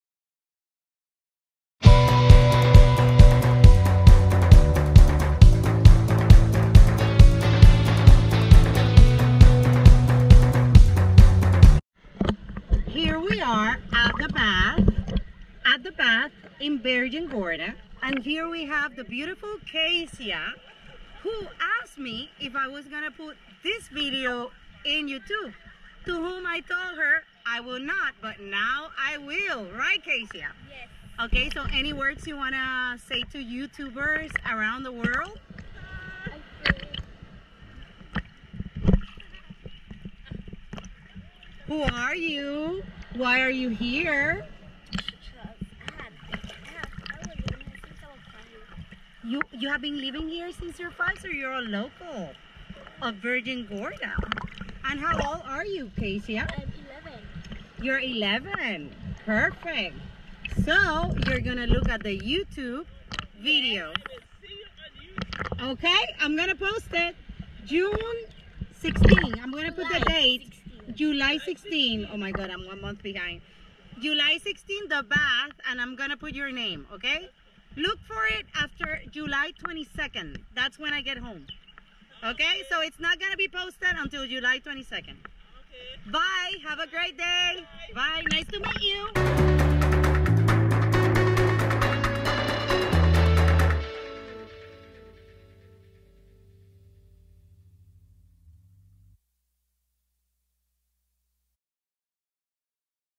Meeting Kaysia at THE BATHS VIRGIN GORDA BVI JULY 16 2019
We met beautiful and happy Kaysia while on a week long sail around the BVI last year.
She and her family are such a great representation of the kindness of the locals in Virgin Gorda, BVI.
We promised her to post this when we got back but life took over...and here we are more than 1 year later; finally learning how to post it and keeping our promise.

If anyone from Virgin Gorda see this and know Kaysia or her mom (in the background providing the approval :) ... let her know... : )